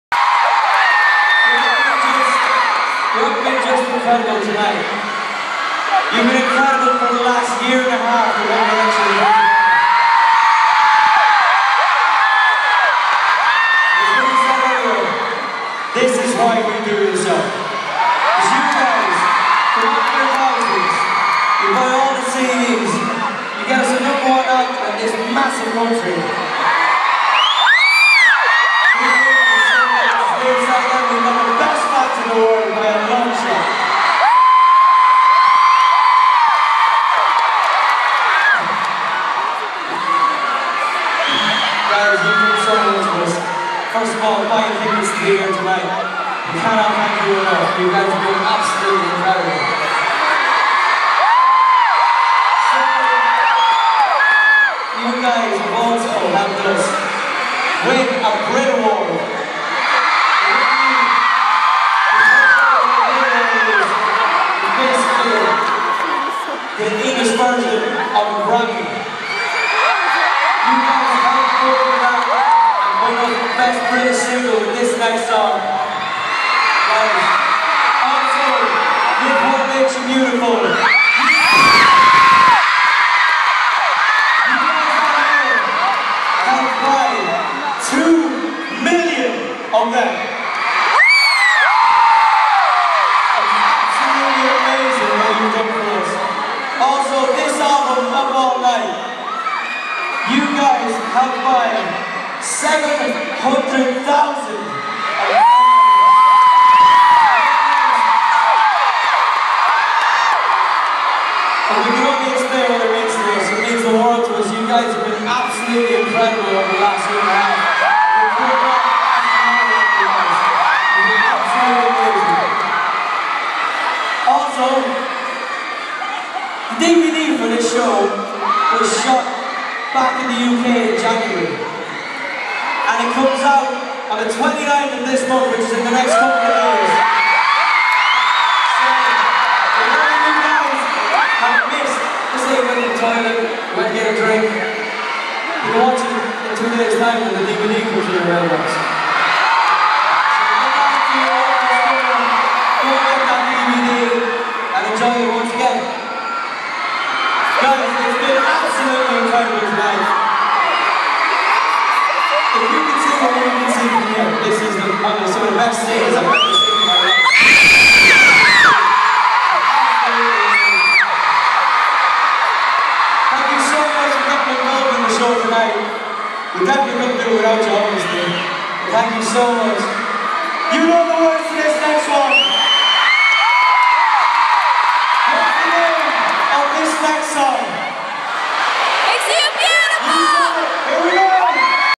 Niall talking
Niall talking about a lot and saying "best gig". 5/28/12 Up All Night Tour